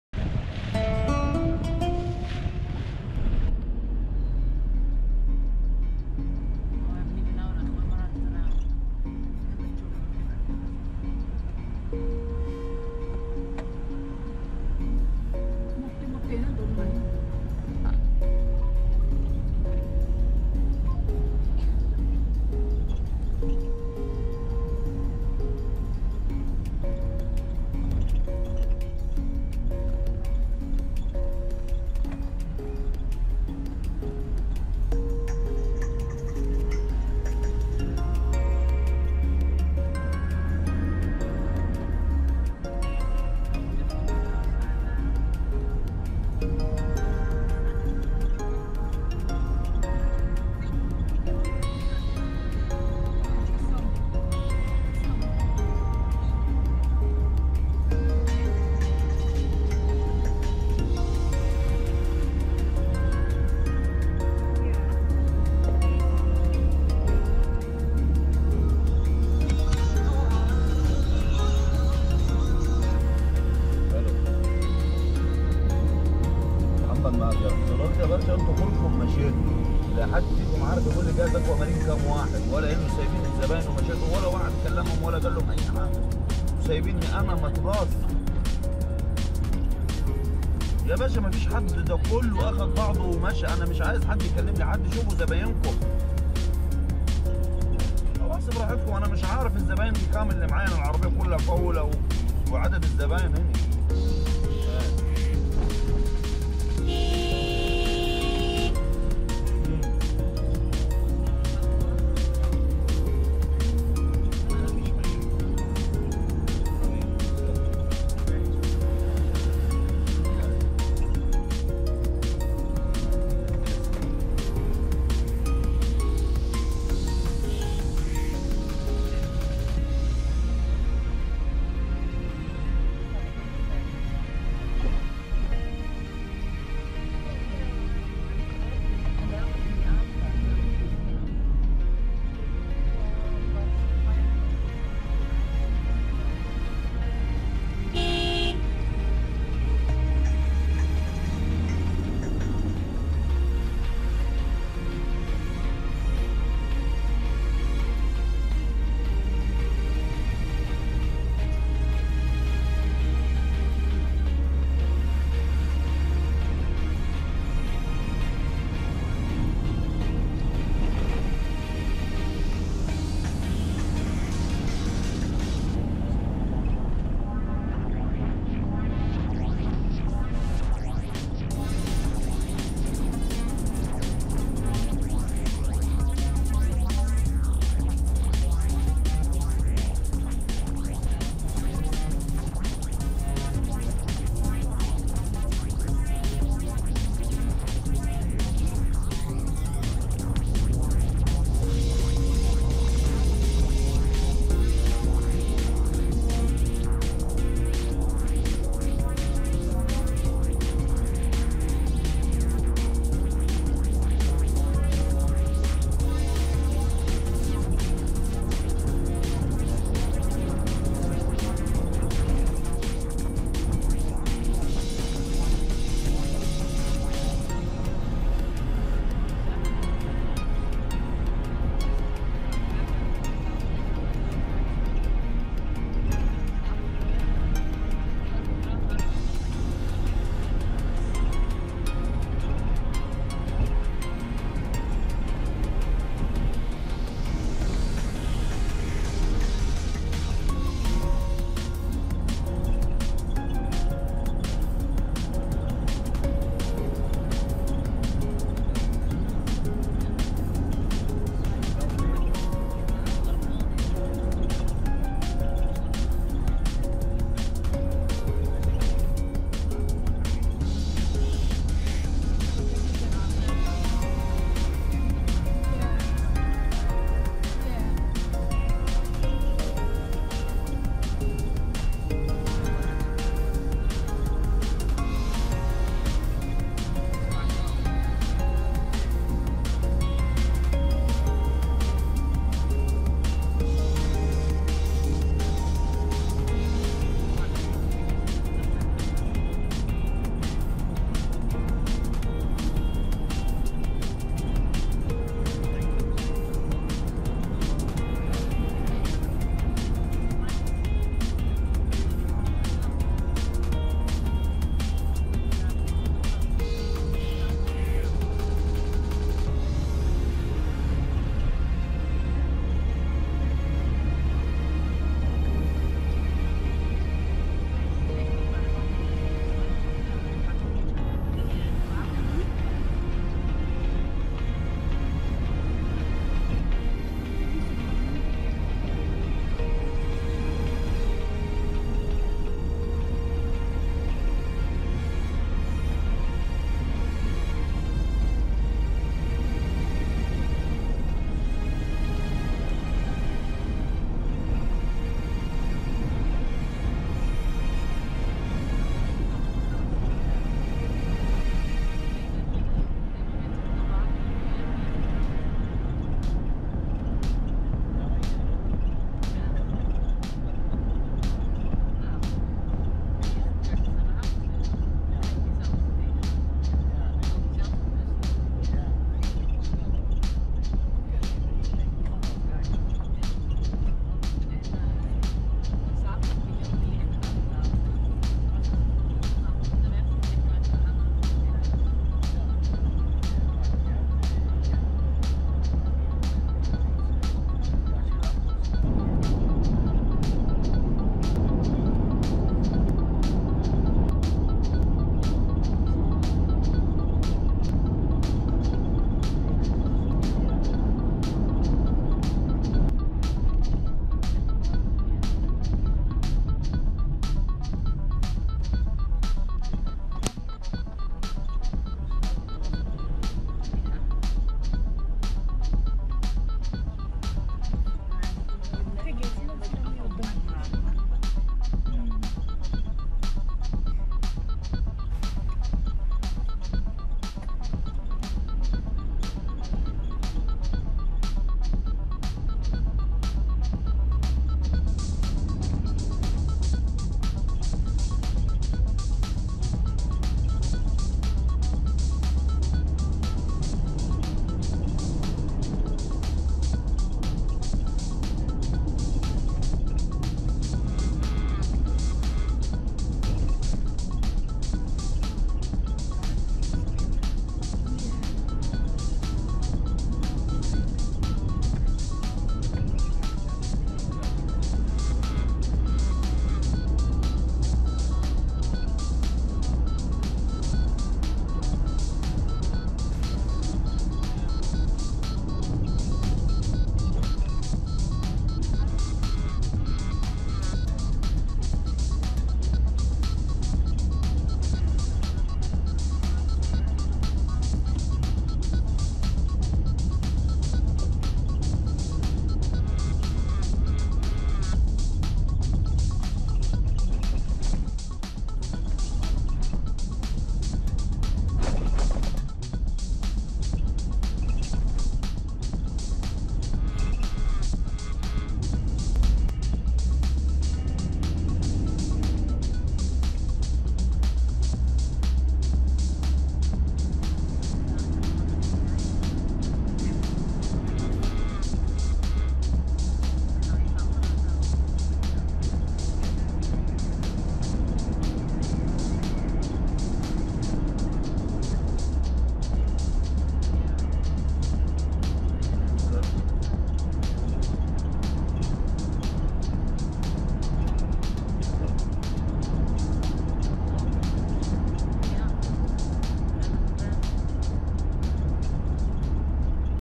Drive through Hurghada, Eygpt
From the Marina to Senzo Mall